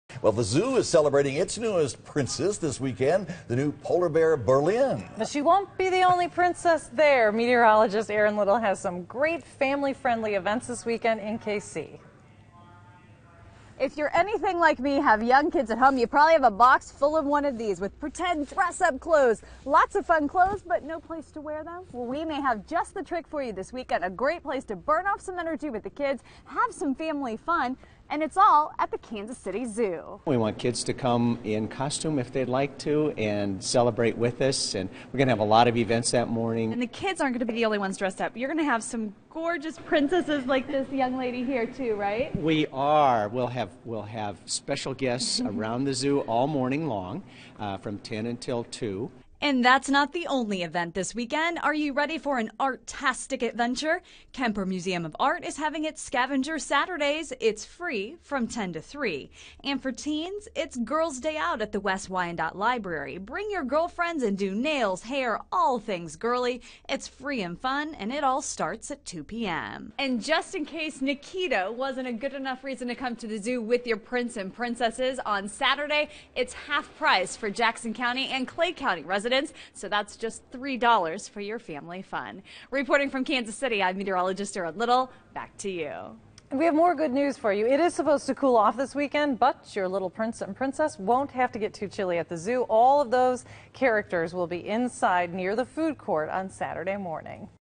Looking for a little bit of fun this weekend?
Princess parties and artistic adventures are just some of the things people can do for a little bit of fun in the Kansas City area this weekend.